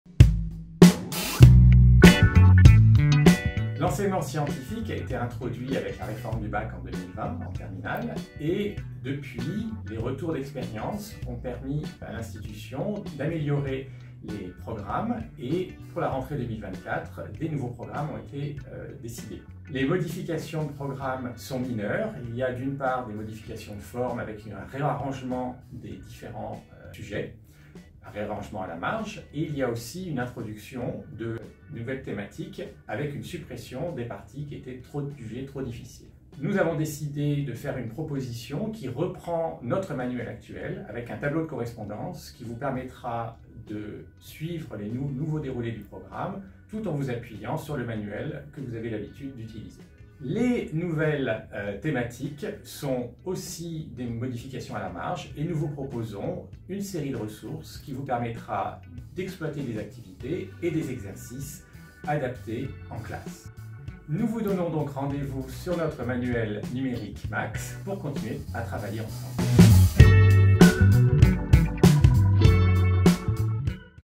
Nouveautés dans votre manuel numérique max d’Enseignement scientifique Terminale
Julien Browaeys, auteur du livre, vous présente les adaptations qui ont été réalisées dans votre manuel numérique max,  en fonction des nouveaux programmes, pour que vous puissiez continuer à l'utiliser.